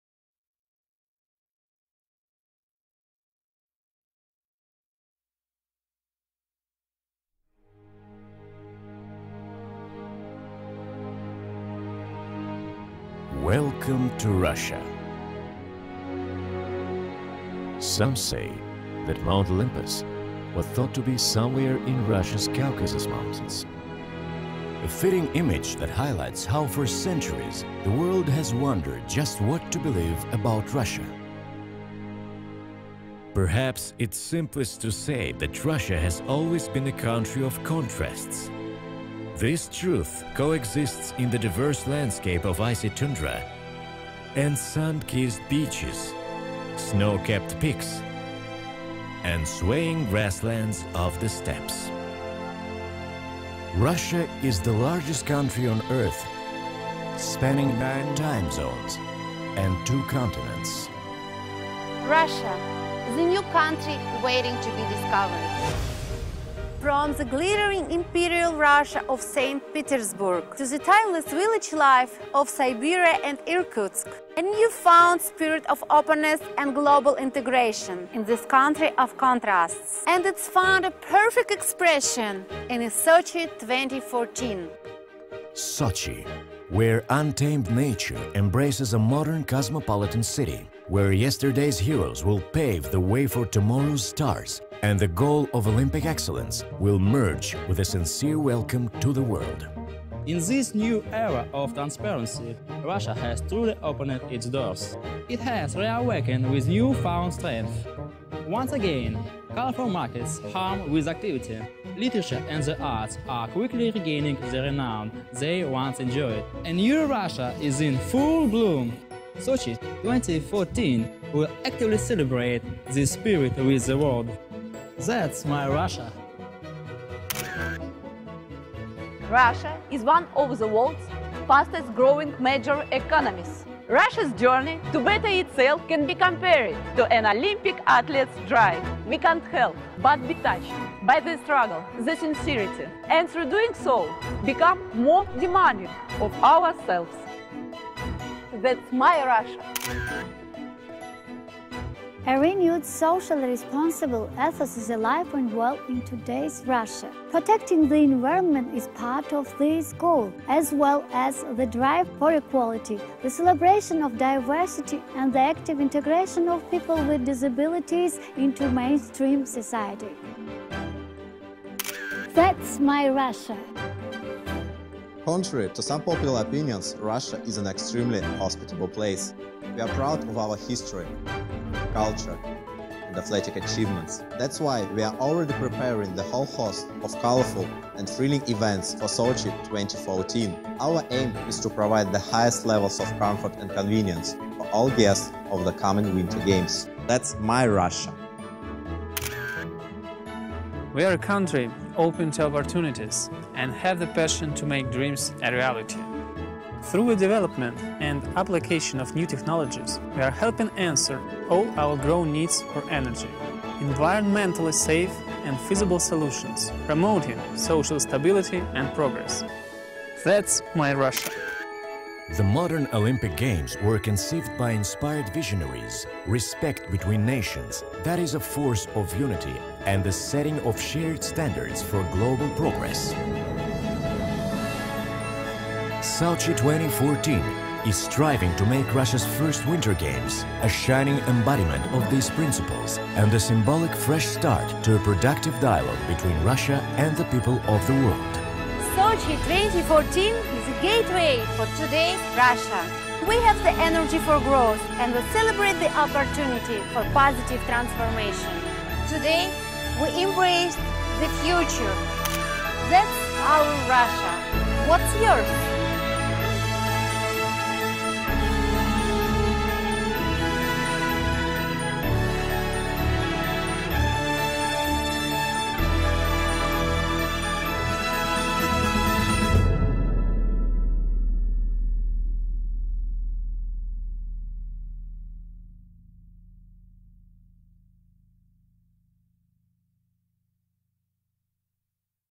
Rosneft - Welcome to Russia (Project Helix)
Information resources, prepared by us for the NK Rosneft, were shown at London Olympics 2012.